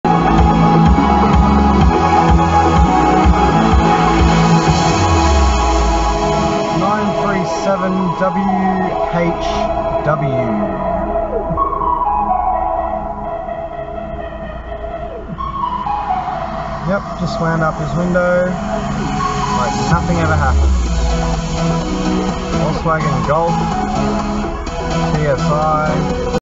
#10 - Littering Incident (Cigarette Butt) - Manly Road
The driver of the vehicle 937WHW can be seen dropping his cigarette butt on the ground out the window of his vehicle.

Don't be a tosser!

Here are some quick facts as to why you shouldn't:
- Approximately 4.5 trillion cigarette butts are littered worldwide every year.
- Approximately 7.2 billion cigarettes are discarded in Australia alone each year. If placed end to end they would extend 144,000 kilometres and circle the planet 3.6 times.
- In most Western countries cigarette butt litter accounts for around 50% of all litter.
- In Australia, over 4,500 fires a year are caused by littered cigarettes.
- Littered cigarette butts can release over 4,000 chemicals into the environment as they disintegrate, contributing to soil and water pollution and impacting wildlife habitat.
- Cigarette filters are made of cellulose acetate, a synthetic fibre derived from wood pulp. They may look like cotton but are actually made of plastic filaments. Meaning they never go away and just turn into microplastics.
- Cigarette filters have been found in the stomachs of fish, birds, sea turtles and other marine creatures.
- Litter is magnetic; the presence of littered cigarette butts tends to encourage others to litter their own.
- As well as the hundreds of other reasons not to litter. There's only one benefit to littering and it's convenience (basically laziness). Other than that, there's not a single positive.